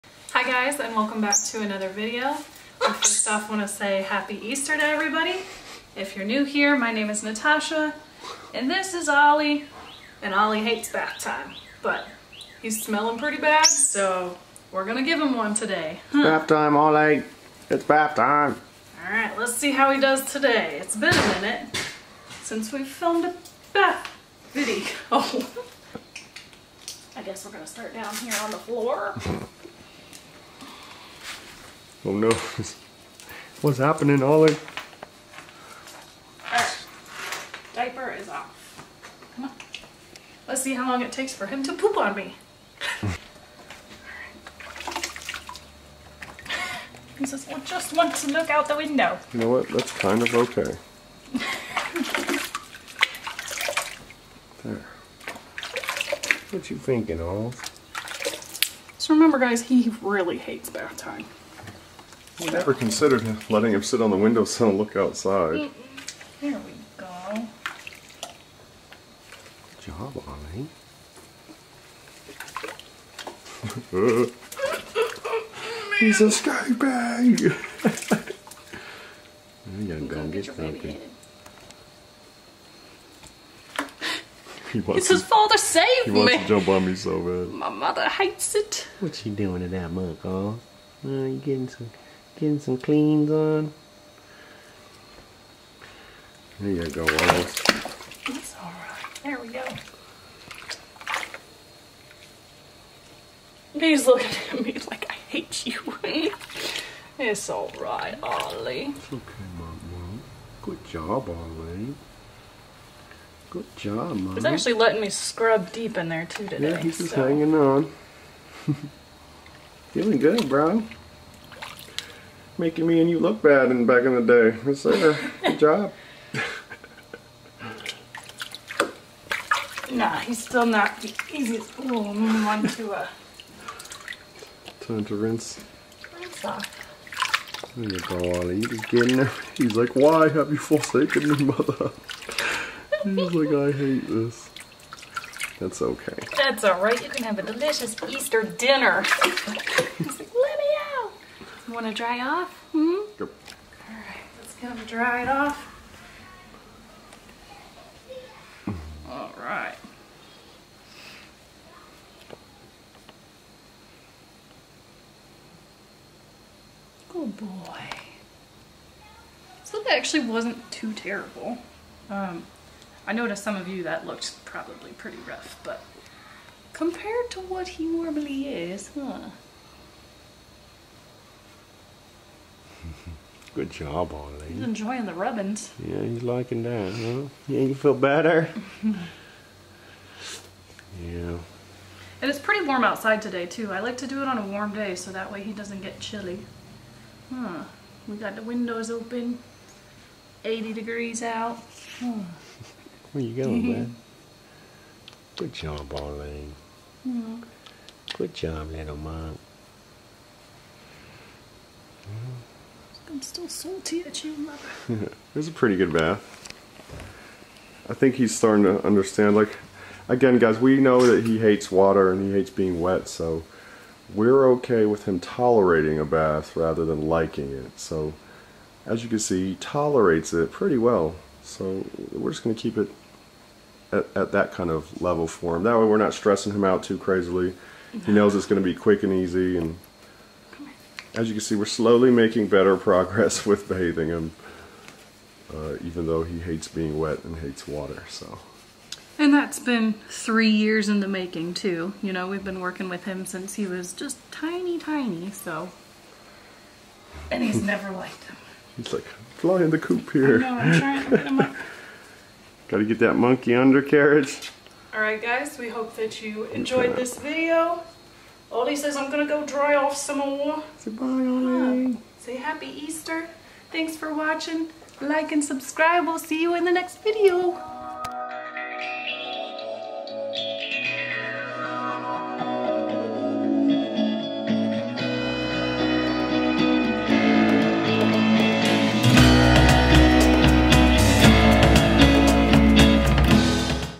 Ollie The Monkey Gets An Easter Bath
HAPPY EASTER
Ollie The Monkey Gets An Easter Bath! We Can't Have A Stinky Monkey For Easter Dinner! Watch Ollie Get A Bath And Dry Off In A Fluffy Towel!

▷oLLie’s AMAZON WISHLIST

SEND oLLie MAIL, CARDS, GIFTS, AND LETTERS
oLLie The Monkey